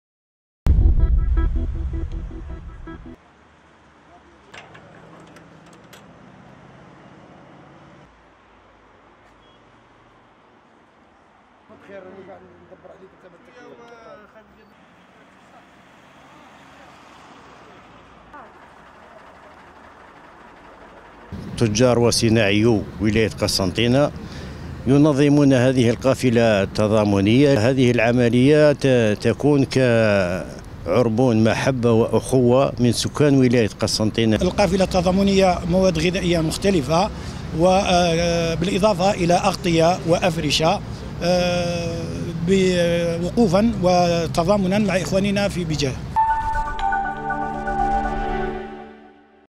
قسنطينة: قافلة تحمل مساعدات غذائية تتجه نحو ولاية بجاية
قسنطينة(وأج)- انطلقت قافلة تضامنية تضم مساعدات غذائية موجهة لفائدة العائلات المتضررة من الزلزال الذي ضرب مؤخرا ولاية بجاية.